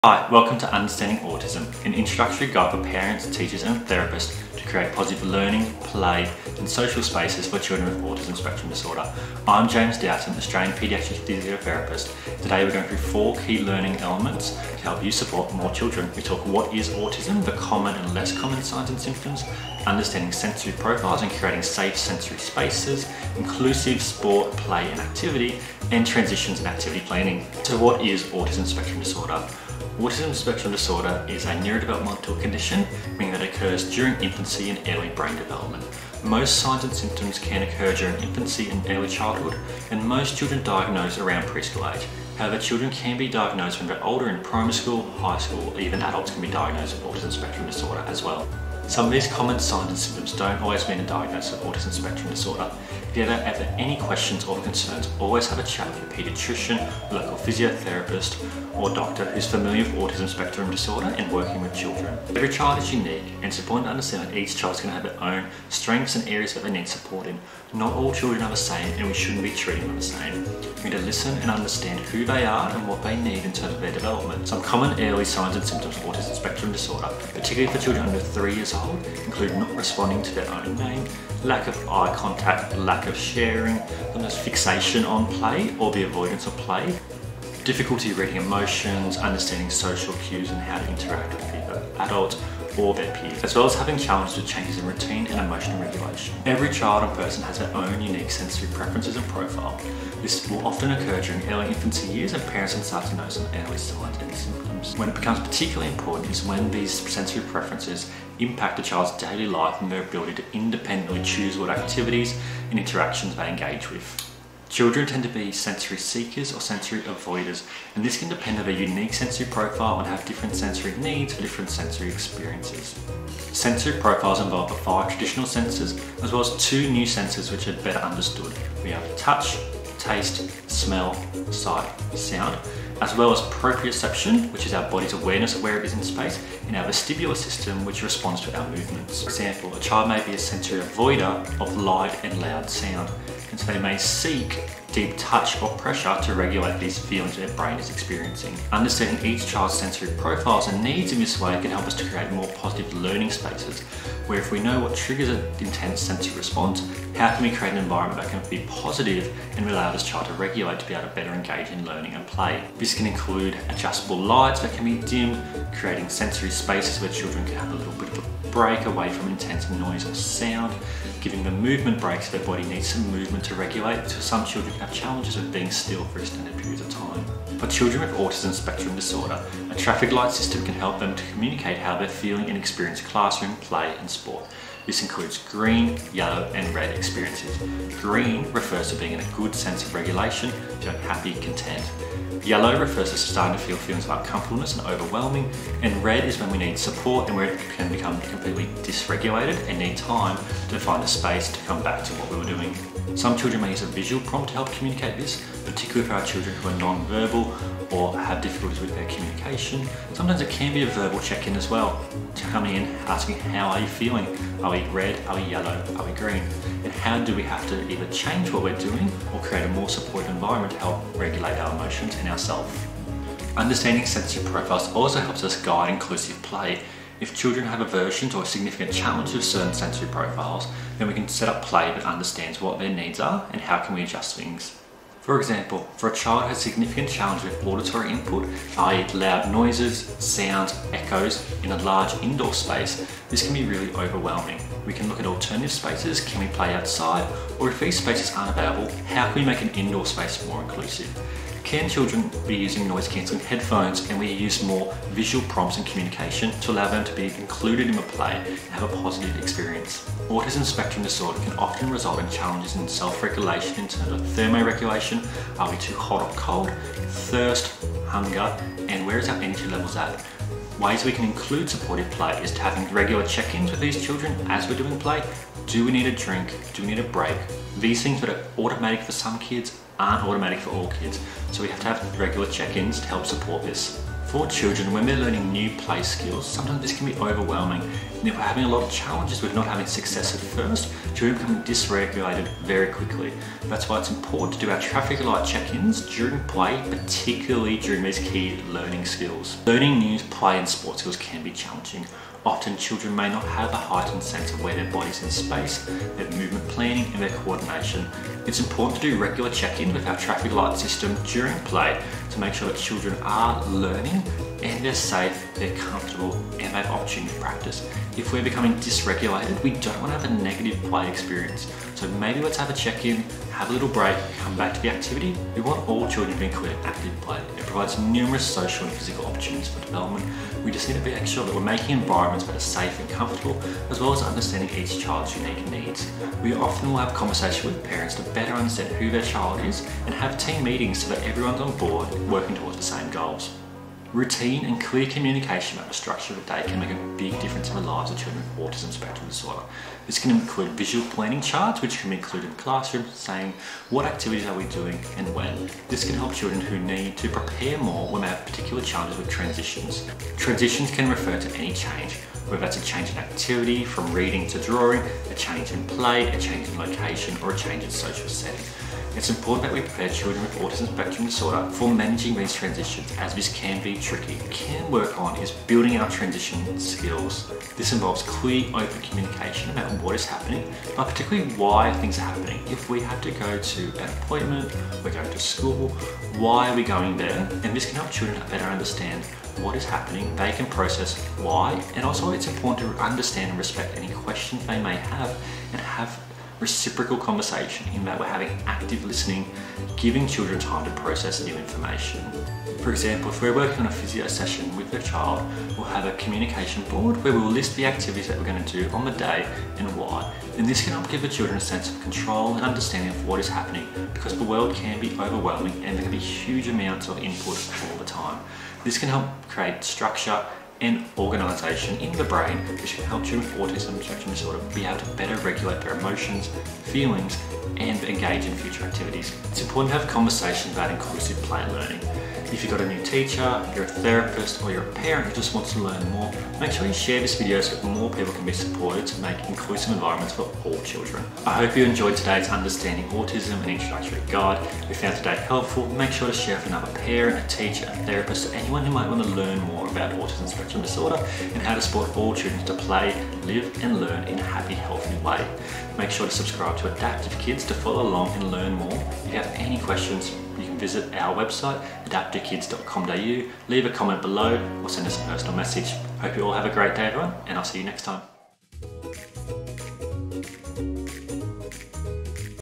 How to teach children with Autism | Preschool + Primary School (From a Paediatric Physiotherapist)
Learn with James Dowton (Australian - Paediatric Physiotherapist) from @adaptivekids  practical steps you can start today for children with Autism Spectrum Disorder.

We discuss:
- What is Autism
- Understanding sensory profiles and needs (Classroom and Learning Tips)
- Inclusive Play and Learning
- Supporting key transitions

All information is general in nature and not personal advice